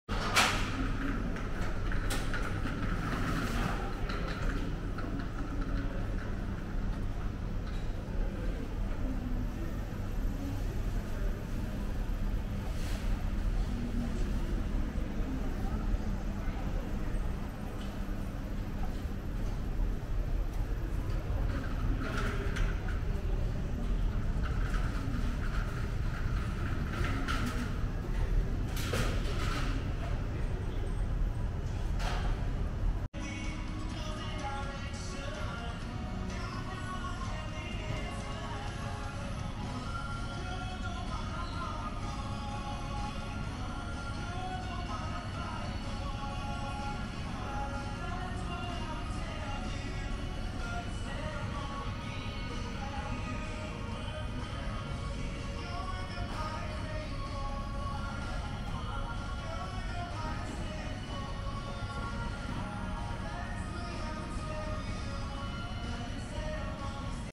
（安定の組み合わせ）はなまるうどんで「牛肉温玉ぶっかけ」を食べて来ました！/ Beef & Egg w/thick broth at Hanamaru Udon
甘辛く煮付けられた牛肉と温玉を絡めて美味しく頂きました。詳細等はブログ内にて。